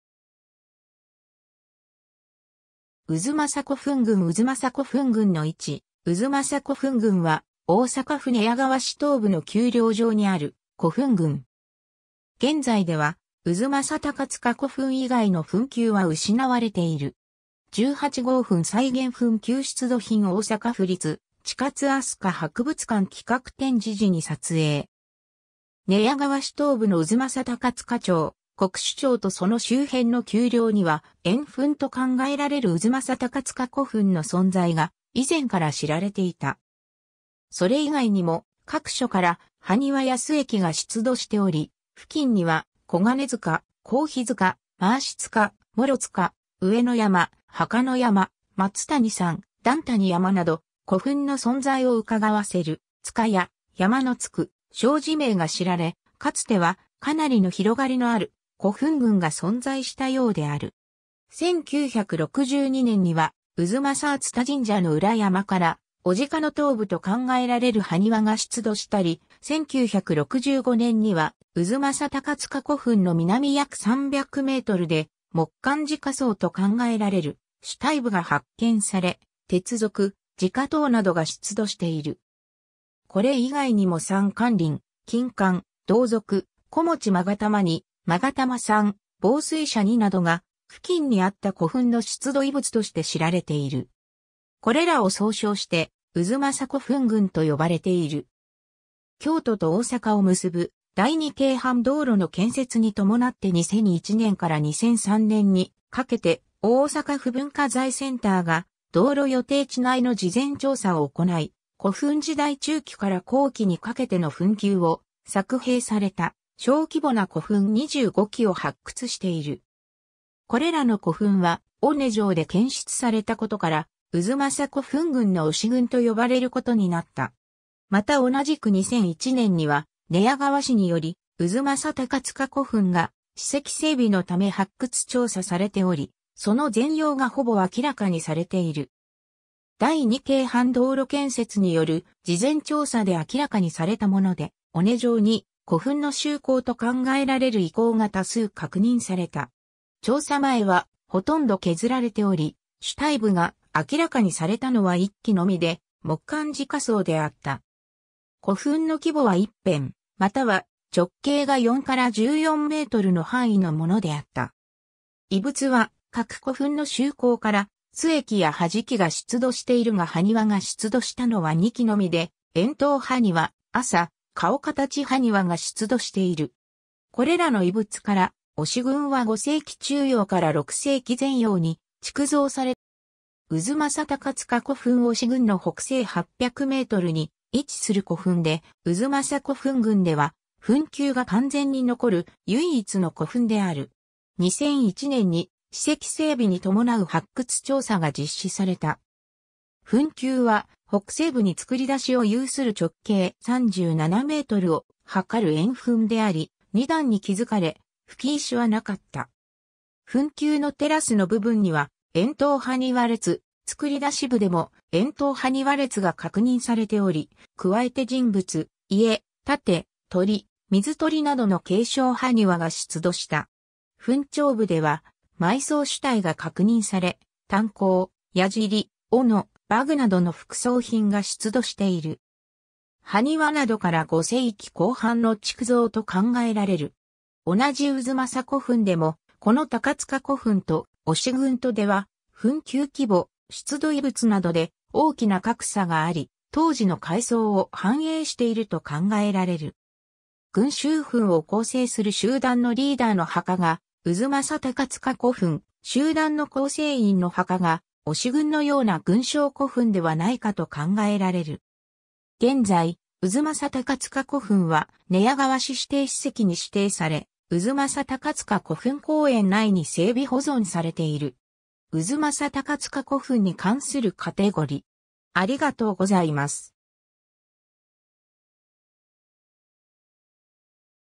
太秦古墳群
太秦古墳群 太秦古墳群の位置 太秦古墳群（うずまさこふんぐん）は、大阪府寝屋川市東部の丘陵上にある古墳群（初期群集墳）。
現在では太秦高塚古墳（寝屋川市指定史跡）以外の墳丘は失われている。
 18号墳 再現墳丘 出土品 大阪府立近つ飛鳥博物館企画展示時に撮影。
 寝屋川市東部の太秦高塚町、国守町とその周辺の丘陵には、円墳と考えられる太秦高塚古墳の存在が以前から知られていた。
それ以外にも各所から埴輪や須恵器が出土しており、付近には小金塚、向ヒ塚、廻し塚、モロ塚、上ノ山、墓ノ山、松谷山、段谷山など古墳の存在を窺わせる「塚」や「山」のつく小字名が知られ、かつては、かなりの広がりのある古墳群が存在したようである。
1962年には太秦熱田神社の裏山から牡鹿の頭部と考えられる埴輪が出土したり、1965年には太秦高塚古墳の南約300メートルで木棺直葬と考えられる主体部が発見され、鉄鏃、直刀などが出土している。
これ以外にも三環鈴、金環、銅鏃、子持勾玉2、勾玉3、紡錘車2などが付近にあった古墳の出土遺物として知られている。
これらを総称して太秦古墳群と呼ばれている。
京都と大阪を結ぶ、第二京阪道路の建設に伴って2001年から2003年にかけて大阪府文化財センターが道路予定地内の事前調査を行い、古墳時代中期から後期にかけての墳丘を削平された小規模な古墳25基を発掘している。
これらの古墳は尾根上で検出されたことから太秦古墳群の尾支群と呼ばれることになった。
また同じく2001年には、寝屋川市により太秦高塚古墳が史跡整備のため発掘調査されており、その全容がほぼ明らかにされている。
 第二京阪道路建設による事前調査で明らかにされたもので、尾根上に古墳の周溝と考えられる遺構が多数確認された。
調査前は、ほとんど削られており、主体部が明らかにされたのは1基（3号墳）のみで木棺直葬であった。
古墳の規模は1辺または直径が4－14メートルの範囲のものであった。
遺物は各古墳の周溝から須恵器や土師器が出土しているが埴輪が出土したのは2基（8・17号墳）のみで円筒埴輪、朝顔形埴輪が出土している。
これらの遺物から尾支群は5世紀中葉から6世紀前葉に築造されたと考えられる。
 太秦高塚古墳 尾支群の北西800メートルに位置する古墳で、太秦古墳群では墳丘が完全に残る唯一の古墳である。
2001年に史跡整備に伴う発掘調査が実施された。
墳丘は北西部に造り出しを有する直径37メートルを測る円墳であり、2段に築かれ、葺石はなかった。
墳丘のテラスの部分には円筒埴輪列、造り出し部でも円筒埴輪列が確認されており、加えて人物、家、盾、鶏、水鳥などの形象埴輪が出土した。
墳頂部では埋葬主体が確認され、短甲、鏃、斧、馬具などの副葬品が出土している。
埴輪などから5世紀後半の築造と考えられる。
同じ太秦古墳でも、この高塚古墳と尾支群とでは墳丘規模、出土遺物などで大きな格差があり、当時の階層を反映していると考えられる。
群集墳を構成する集団のリーダーの墓が太秦高塚古墳、集団の構成員の墓が尾支群のような群小古墳ではないかと考えられる。
 現在、太秦高塚古墳は寝屋川市指定史跡に指定され、「太秦高塚古墳公園」内に整備保存されている。
 太秦高塚古墳に関するカテゴリ: